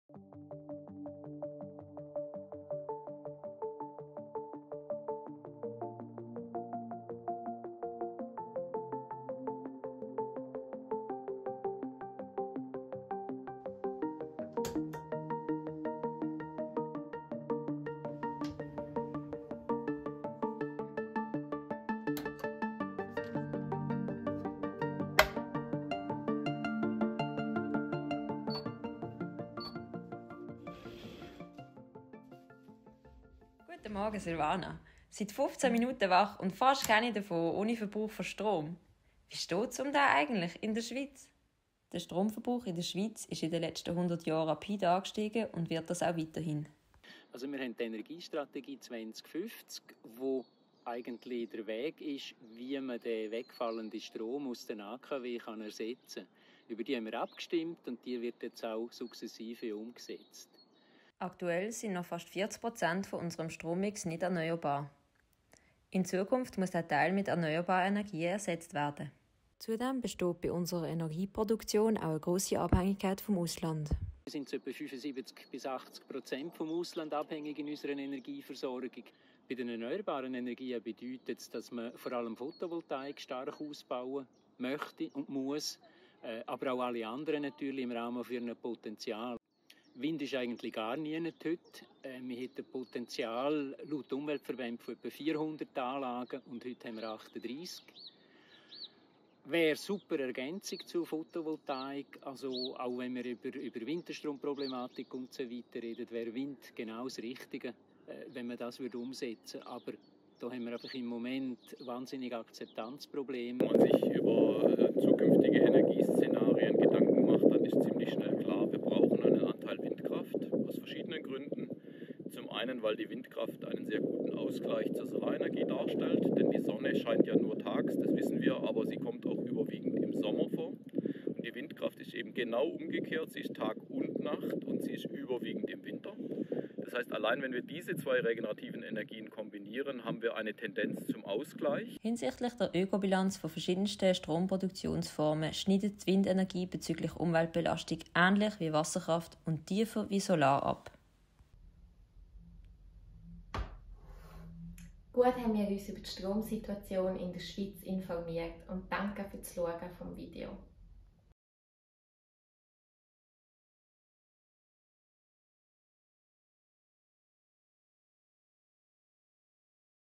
Alles was du nicht über die Schweizer Energieversorgung weisst
Toll, dass Du da bist & Dich über Windenergie informierst.
In diesem kurzen Video erfährst du die wissenswertesten Fakten zur Stromversorgung in der Schweiz und was es mit der erneuerbaren Windenergie auf sich hat.
Nachdem Du dir dieses Video angeschaut hast, kannst du selbstsicher bei Gesprächen und Diskussionen über die Stromversorgung- und politik mitreden!
Dieses Video wurde als ein Unterrichtsprojekt von Studierenden des Bachelors Umweltingenieurswesen der ZHAW im Rahmen des Moduls «Umweltkommunikation» erstellt. Produzentinnen: Silvana C. / Kim J.